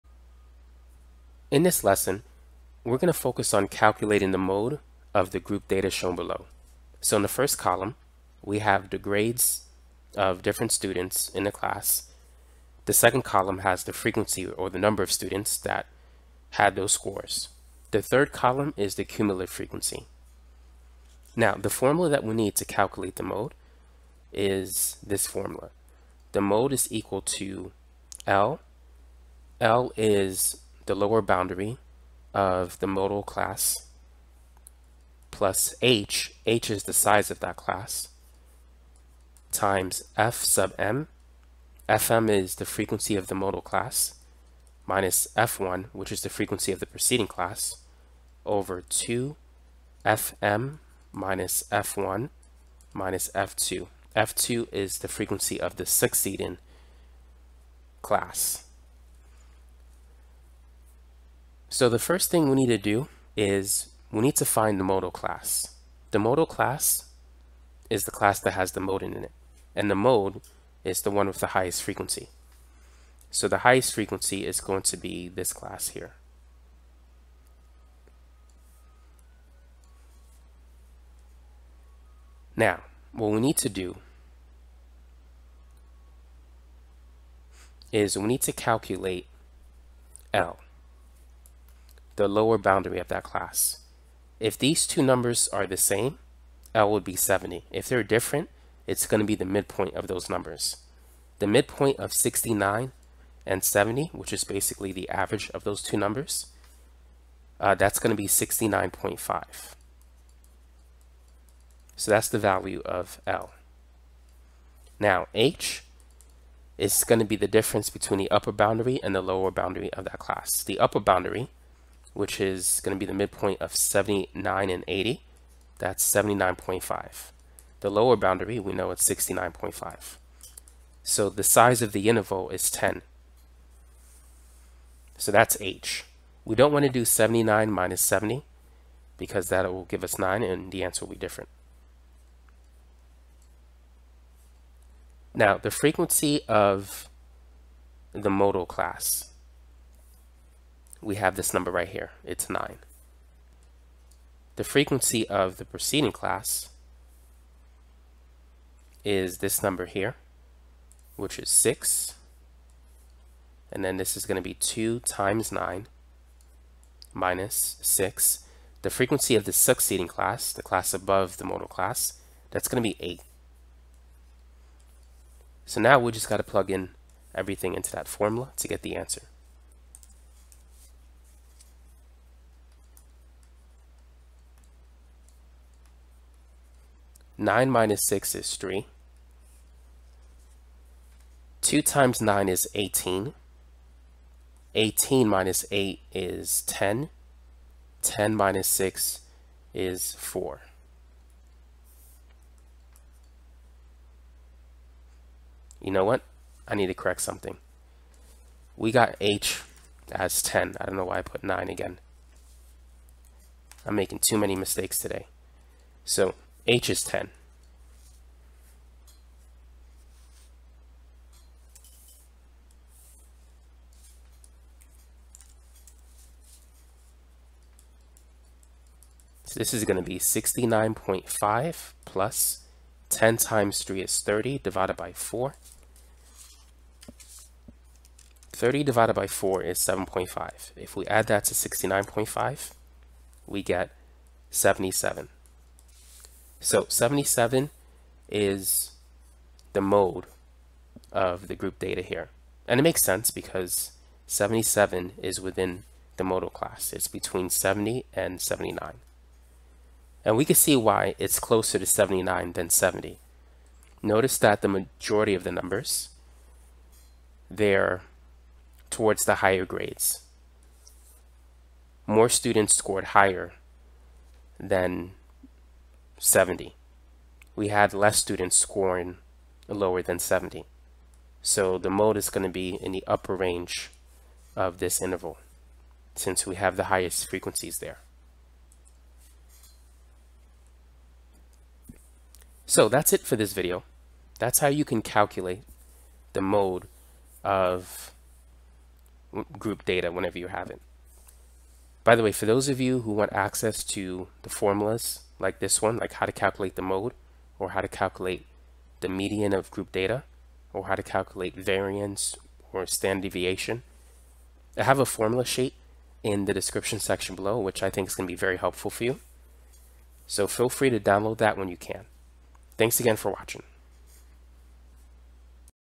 How To Calculate the Mode of Grouped Data - Statistics
This statistics video tutorial explains how to calculate the mode of grouped data using a formula.